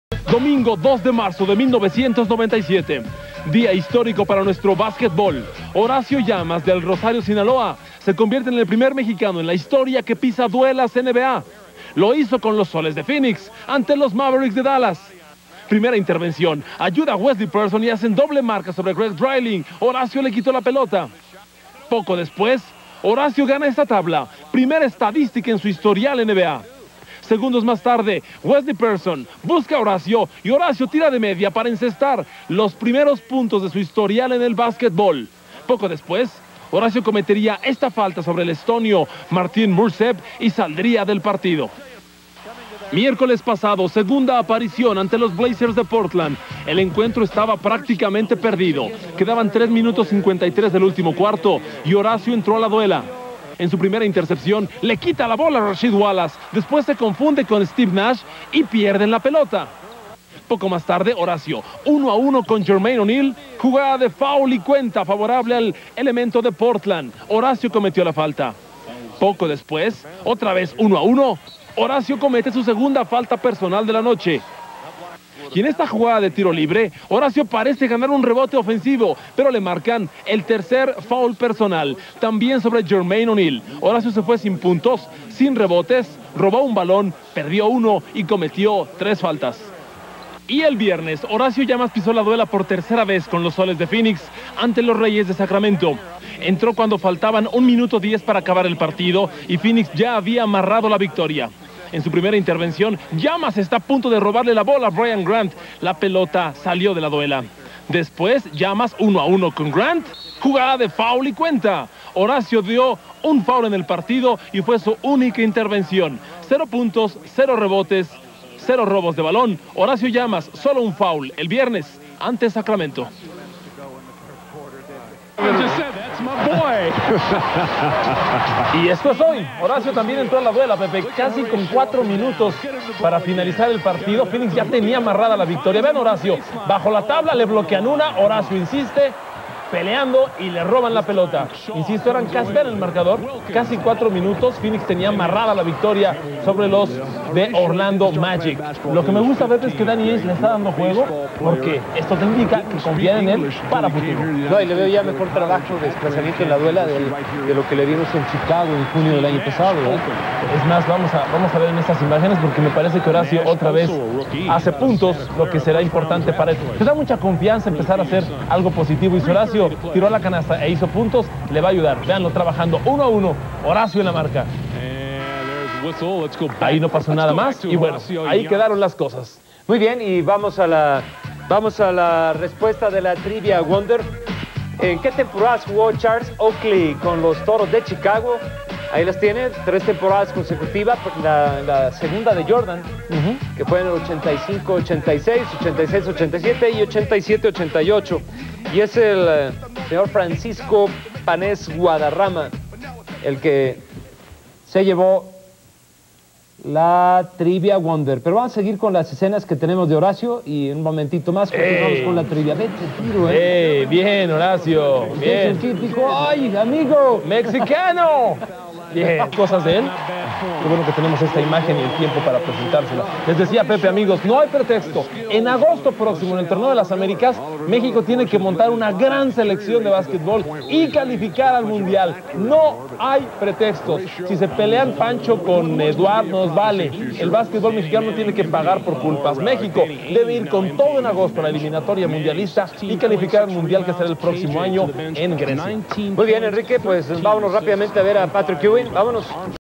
Reportaje sobre Horacio Llamas y su ingreso a la NBA (1997).
En 1997, el basquetbolista Horacio Llamas ingresa a la NBA al equipo de los Phoenix, Suns, convirtiéndose en el primer mexicano en pertenecer a esta liga. Aquí un reportaje  sobre sus primeros pasos en el básquetbol estadounidense, dentro de una transmisión dominical de baloncesto, en Tv7 XHIMT.

Este video como todos los contenidos de este canal son subidos a la red con la única finalidad de compartirlos con la comunidad.